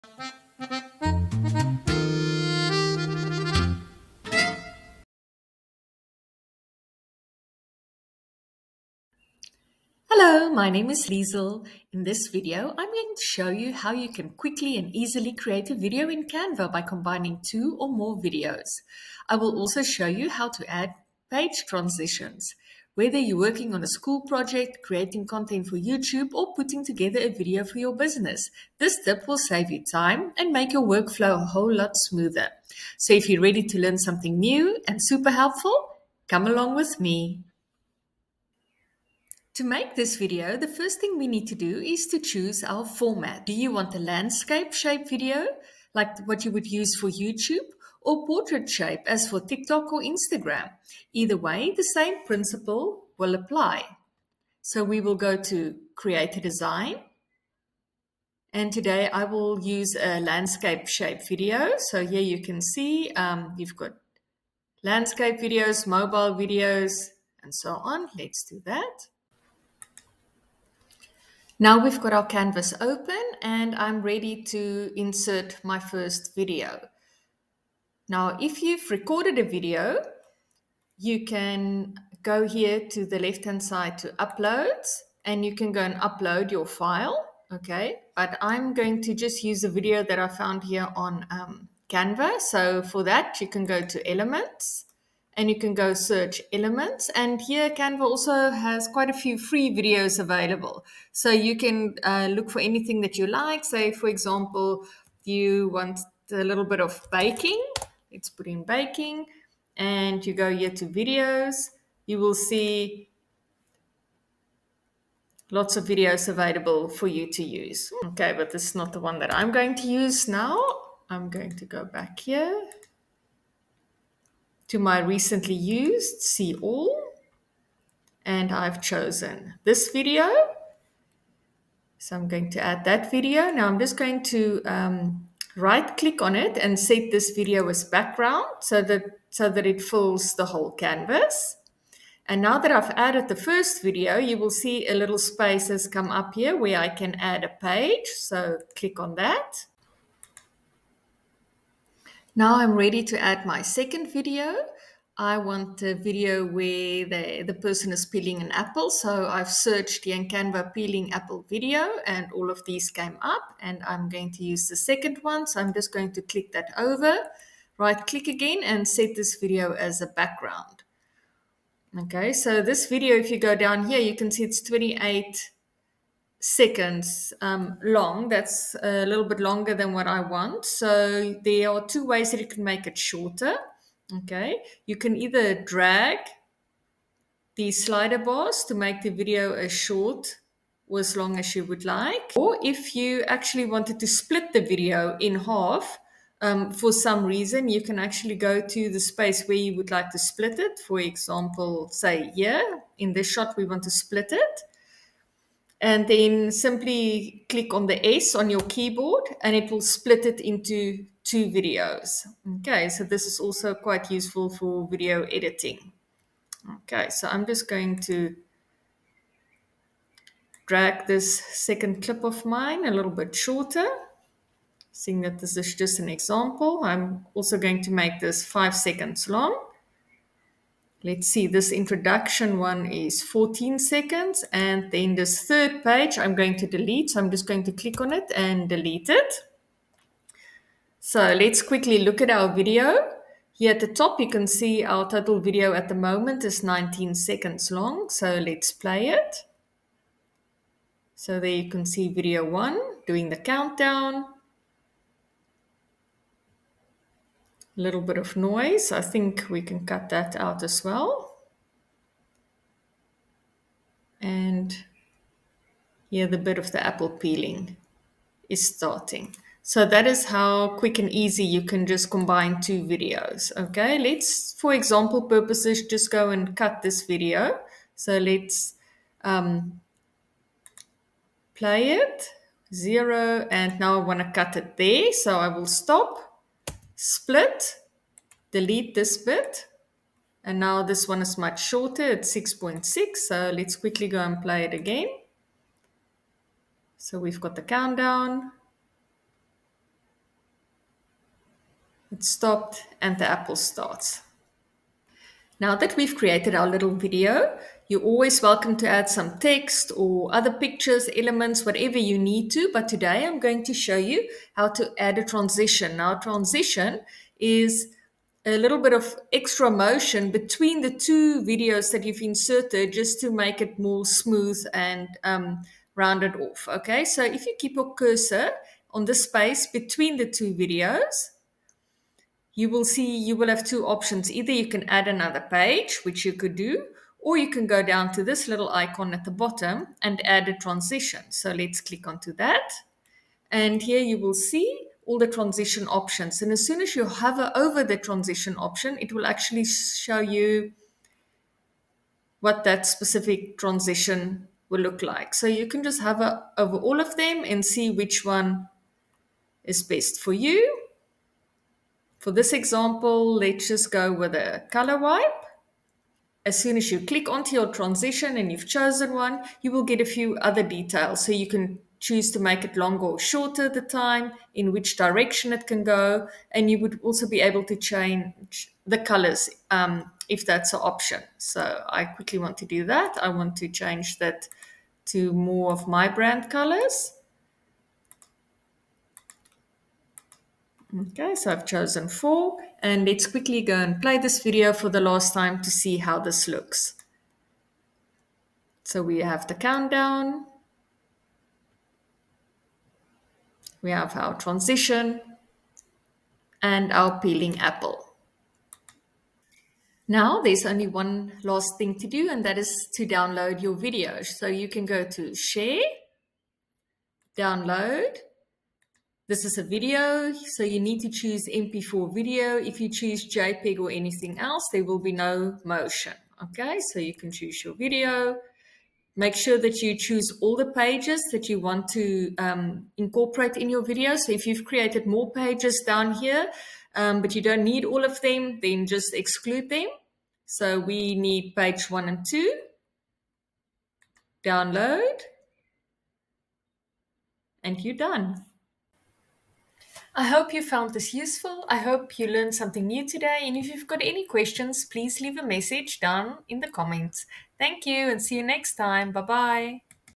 How to combine Two Videos in Canva | Canva Video EditingTutorial #43
In this video, I will show you how to quickly and easily create a video in Canva by combining two or more videos. I will also show you how to add page transitions. Whether you're working on a school project, creating content for YouTube, or putting together a video for your business, this tip will save you time and make your workflow much smoother. So if you’re ready to learn something new and super helpful, come along with me!"

❊ VIDEO CHAPTERS
00:00 Intro
00:38  Tutorial Start
05:05  Add a page transition